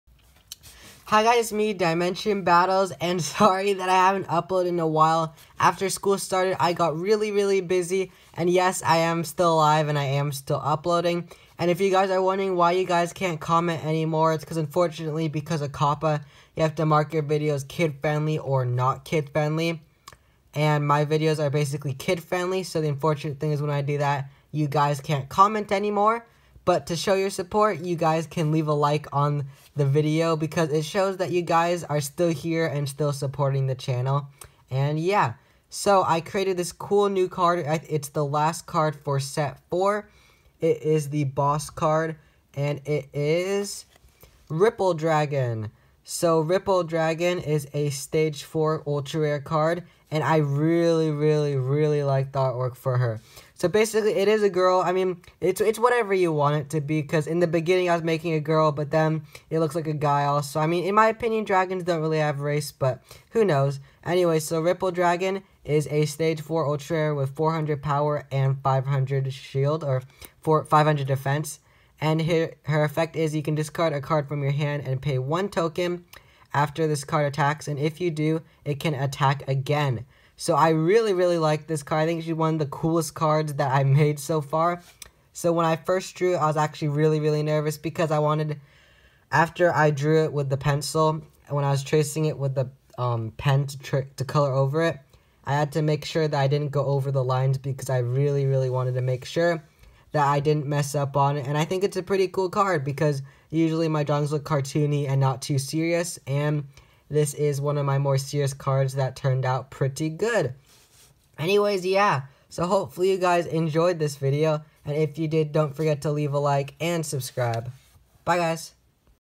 Homemade tcg: Ripple Dragon (Dimension Battles)
Hope you enjoy this homemade tag video where I show the last card of set 4, Ripple Dragon!